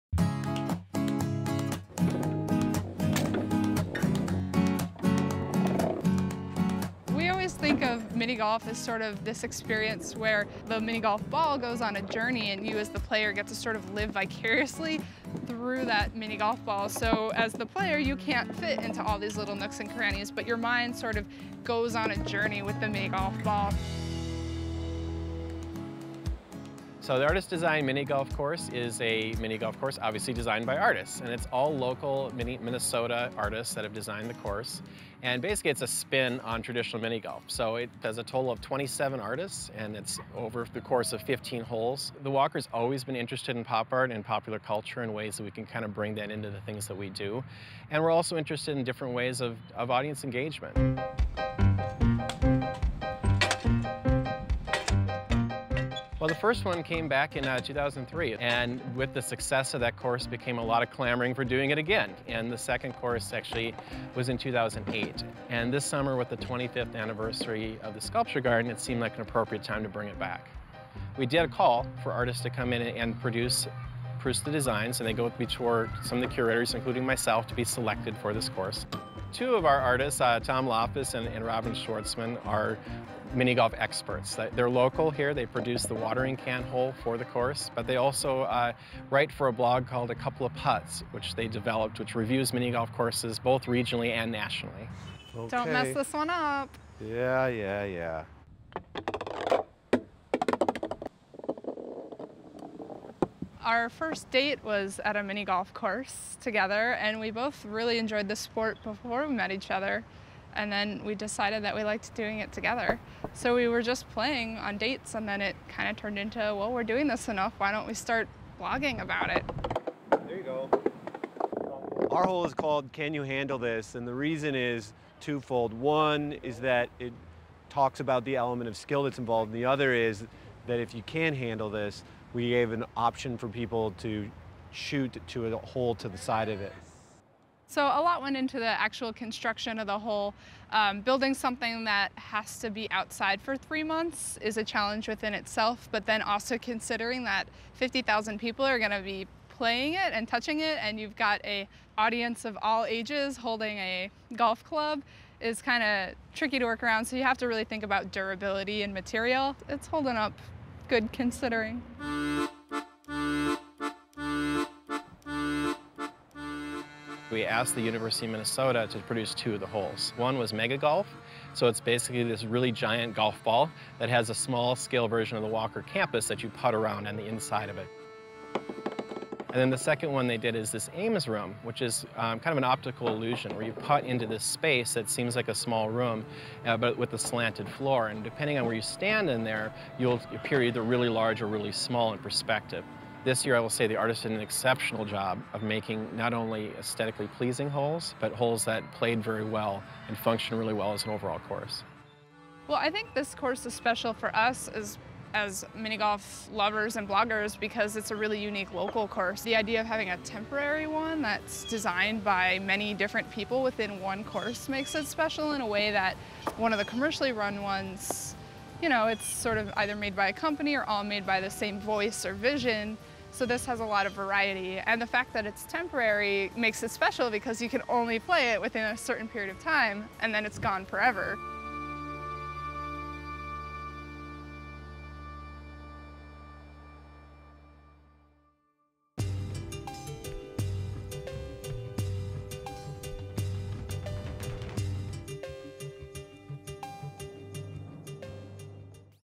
Walker Mini Golf
The Walker Art Center's 2013 Artist-Designed Mini Golf commemorated the 25th Anniversary of the Minneapolis Sculpture Garden.

Photos courtesy of: Walker Art Center
Special Thanks: Walker Art Center